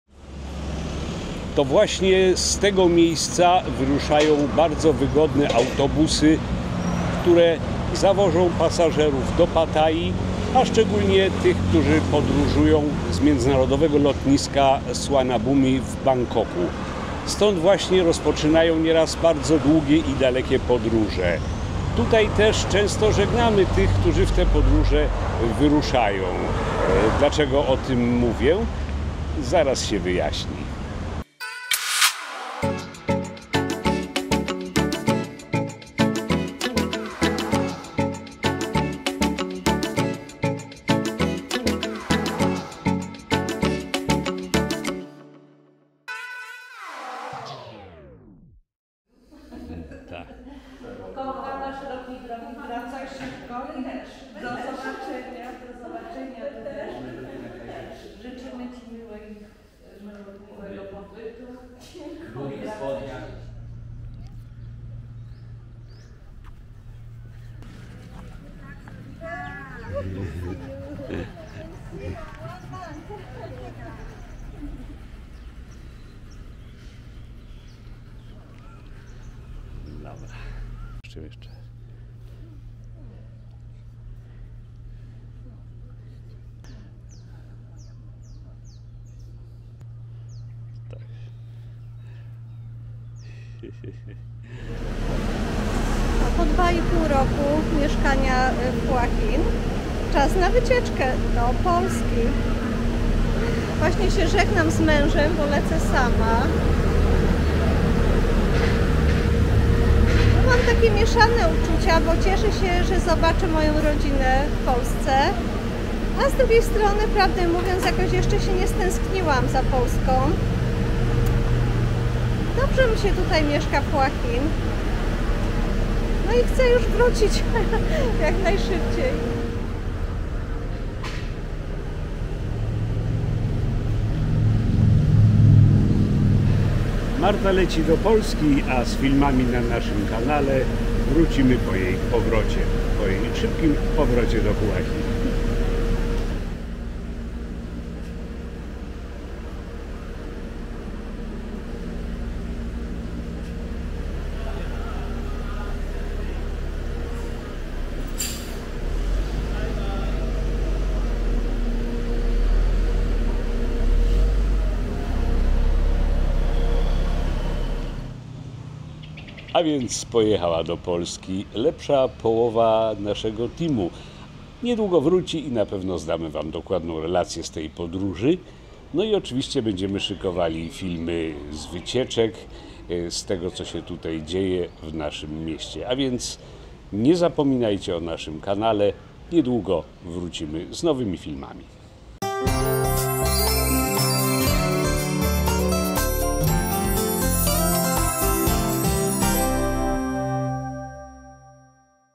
119. Bangkok, Pekin, Warszawa, Kraków - długa droga do rodzinnego miasta.
Niektórzy mówią, że po 2 latach pobytu na obczyźnie tęski się do rodzinnego kraju i miasta. Czy w naszym przypadku tak jest? Trudno powiedzieć, tu życie toczy się inaczej i absorbuje nas całkowicie. Obecna technika pozwala na łatwy kontakt (nawet filmowy) z bliskimi, choć oczywiście nie zastąpi to kontaktu w realu.
Jednak kiedy nadarza się okazja odbycia dalekiej podróży do ojczystego kraju - trzeba z niej skorzystać.
Marta - czyli filmowa Grażyna wybrała się w odwiedziny do rodziny - spędzi Wielkanoc z bliskimi. Ja zostaję "na gospodarstwie".
Po powrocie Marty z Polski będziemy kontynuować przygotowanie nowych filmów na naszym kanale.